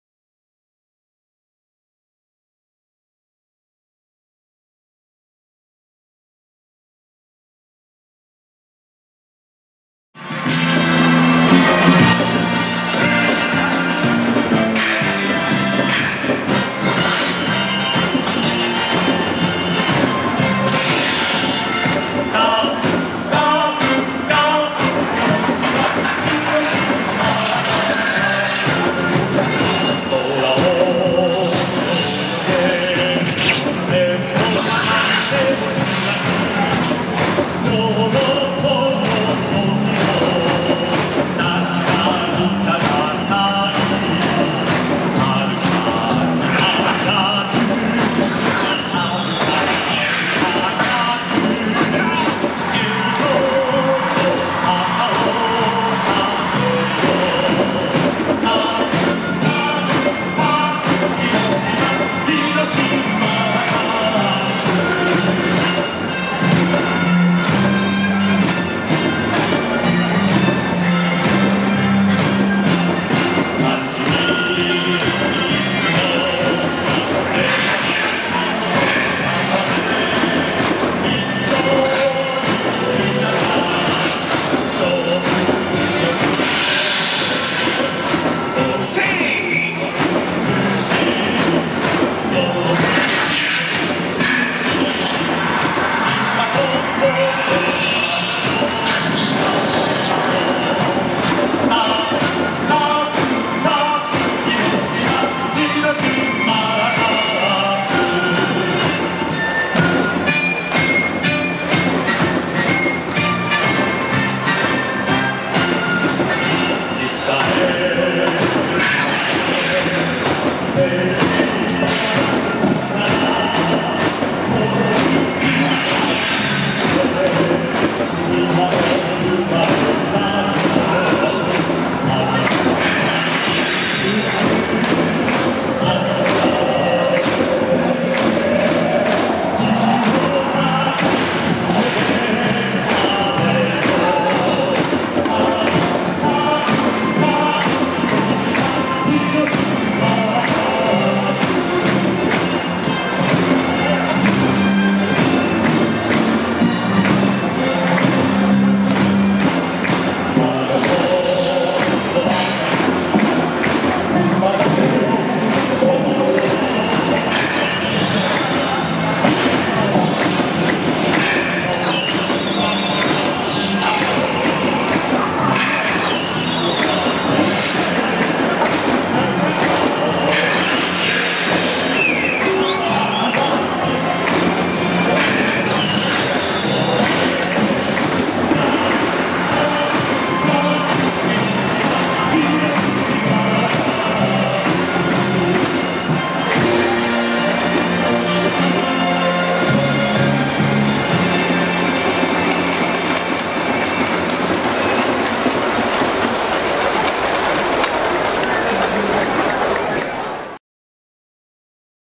080607 マスコット交流 スラィリー＆ネプリプ＠広島市民球場
2008年6月7日（土）広島市民球場で開催されたセ・パ交流戦の広島カープvsオリックス・バファローズ。マスコット交流でスラィリーと共にカープ応援歌に合わせて踊るネッピー＆リプシー。